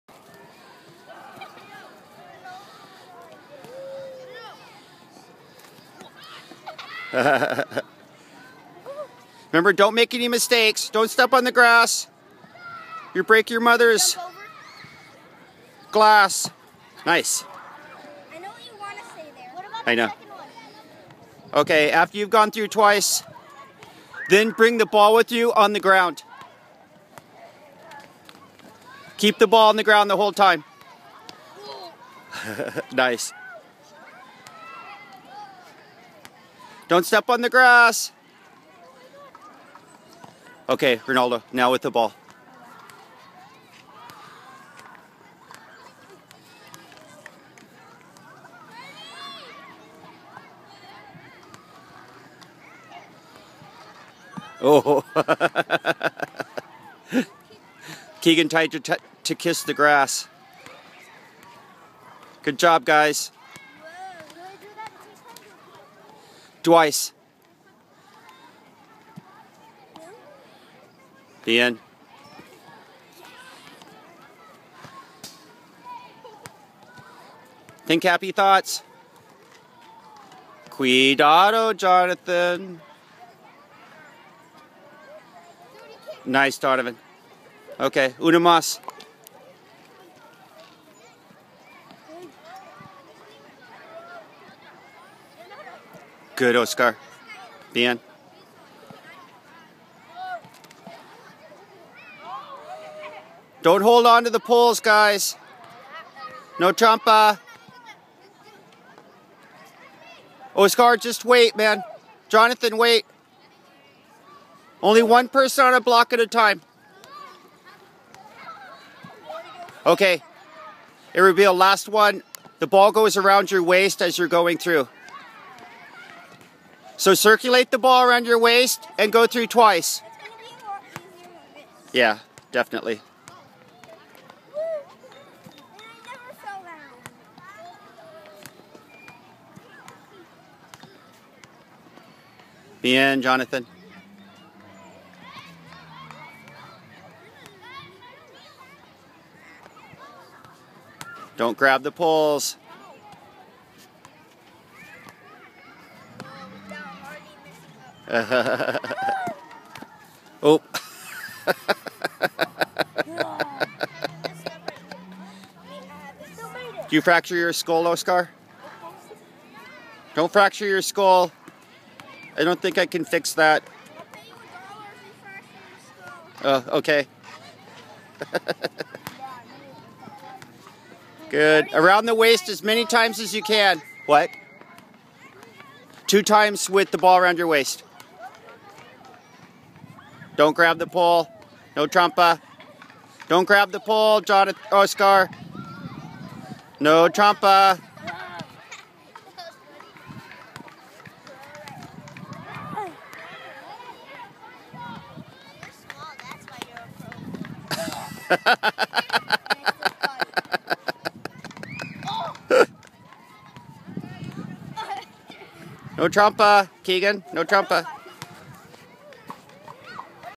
Creative Soccer Instruction - Balance, Agility and Coordination
Fiorentina youth academy based coordination training to improve balance, combining and coupling movement to improve coordination, and develop better agility by stepping over or ducking under the tape. The challenge was increased by having the players dribble a futsal ball with the bottom of their feet as they walked across the balance blocks, and additional challenge can be found by circulating a ball around their waist while walking across the blocks.